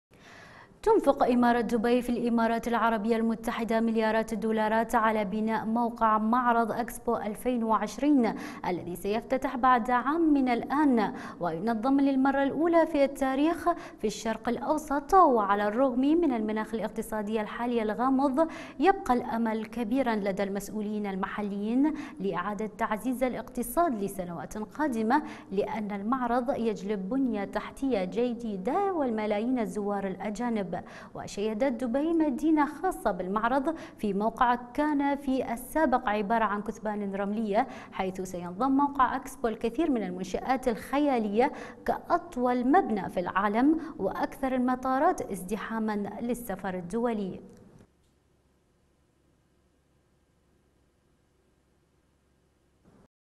إكسبو 2020 دبي تبني موقعا خياليا لإقامة المعرض
إكسبو 2020
دبي تبني موقعا خياليا لإقامة المعرض

تنفق إمارة دبي في الإمارات العربية المتحدة مليارات الدولارات على بناء موقع معرض "إكسبو الفين وعشرين " الذي سيفتتح بعد عام من الآن وينظم للمرة الأولى في التاريخ في الشرق الأوسط , وعلى الرغم من المناخ الاقتصادي الحالي الغامض يبقي الأمل كبيرا لدى المسؤولين المحليين لإعادة تعزيز الاقتصاد لسنوات قادمة لأن المعرض يجلب بنية تحتية جيدة وملايين الزوار الأجانب. وشيدت دبي مدينة خاصة بالمعرض في موقع كان في السابق عبارة عن كثبان رملية، حيث سيضم موقع إكسبو  الكثير من المنشآت الخيالية كأطول مبنى في العالم وأكثر المطارات ازدحاماً للسفر الدولي.